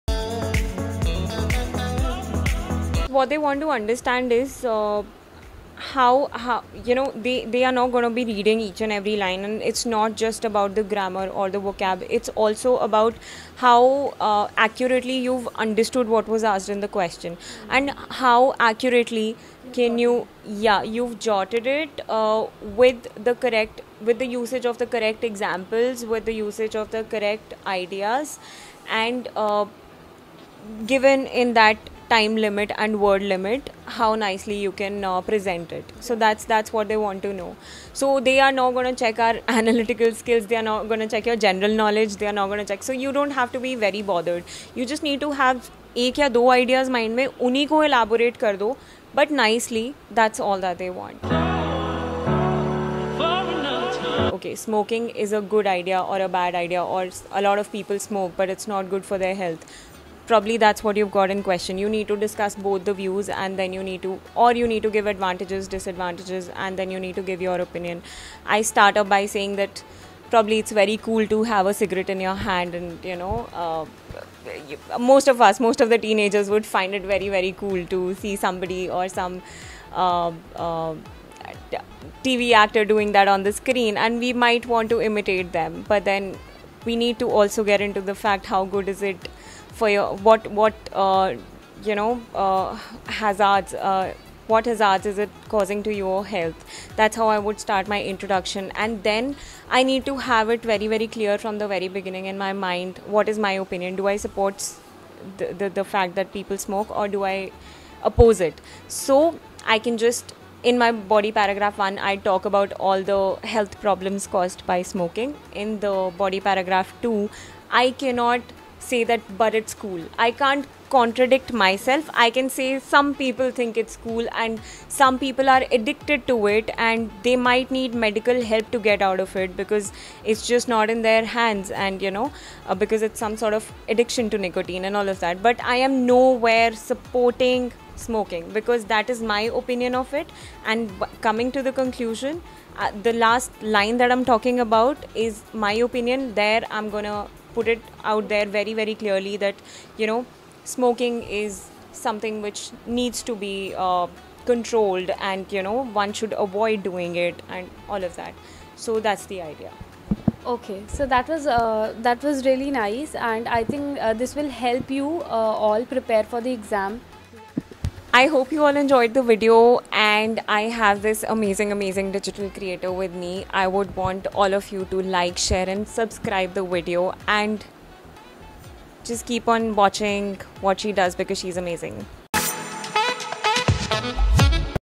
IELTS Writing Guaranteed Success (Part 4) | What Examiners Check in IELTS Exam
This IELTS Writing Series is what you need to watch to score the desired IELTS Result.

Why I have guaranteed the IELTS Writing (Task 2) success because this series is absolutely what you need to hear. This is the 1st part of the series and this video will tell you that

WHY YOU ARE NOT ABLE TO SCORE above 6.5 Bands in IELTS?

If in case you implement what all is told in the series in your exam. You will definitely get the desired score.